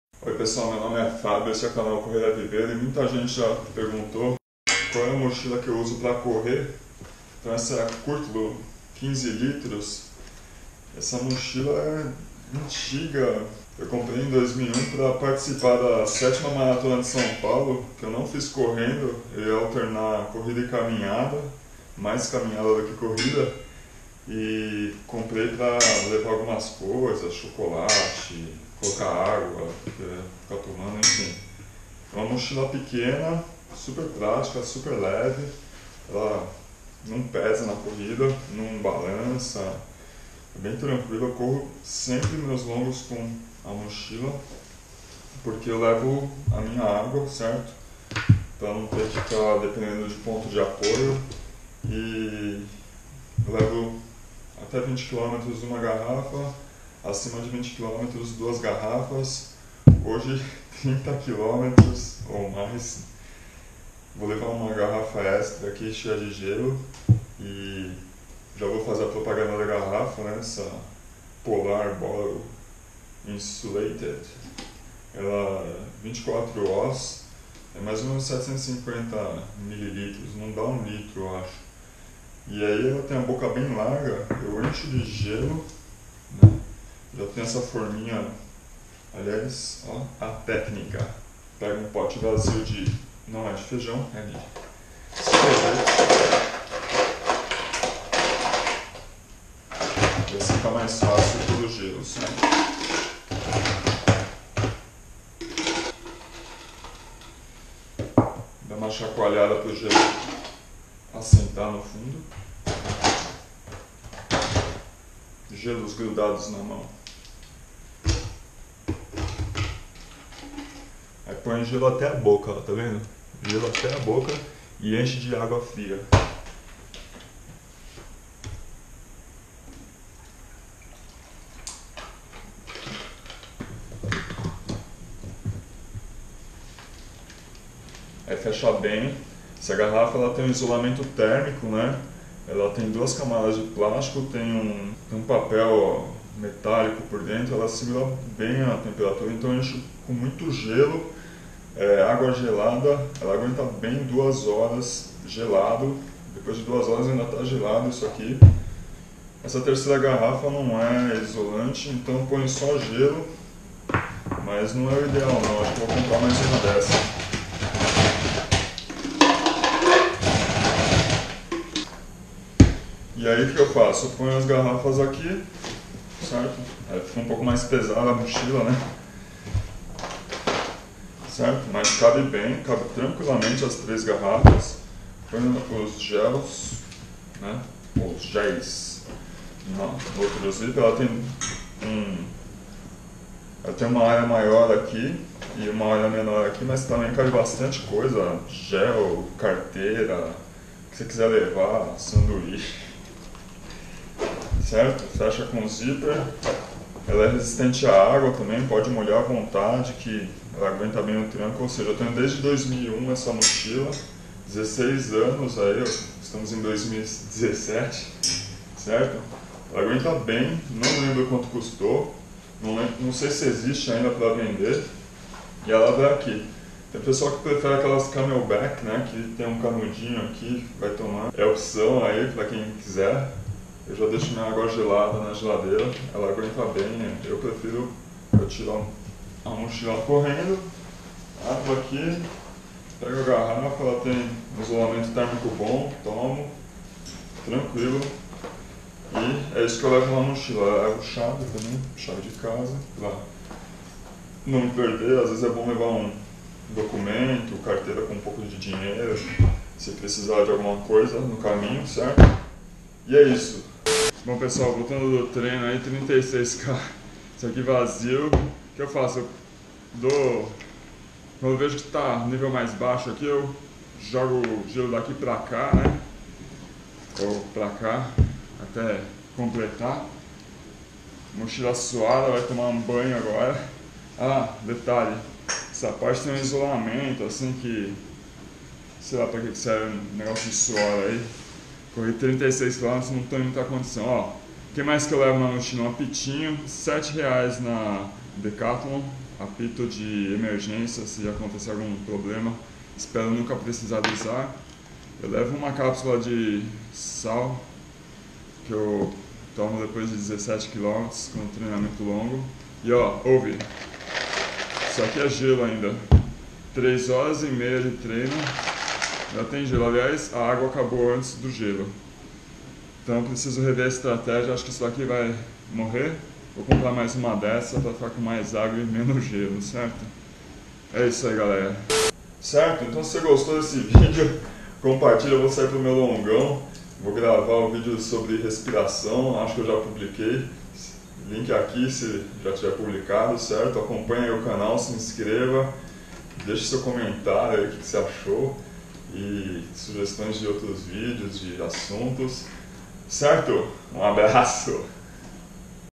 Review: mochila curtlo e garrafa polar
Atendo a pedidos, mostro a mochila que uso nos meus treinos longos. Aproveito para mostrar o que levo dentro dela: garrafas cheias de gelo e água gelada, gel carboidrato, cápsula de sal, apito de emergência.
Mochila Curtlo 15L
Garrafa Polar Bottle Insulated  24oz (24 onças ~ 710mL) com isolamento térmico (garrafa térmica de plástico com papel metálico)
Essa mochila eu comprei em 2001, não lembro quanto custou e não sei se ainda vendem esse modelo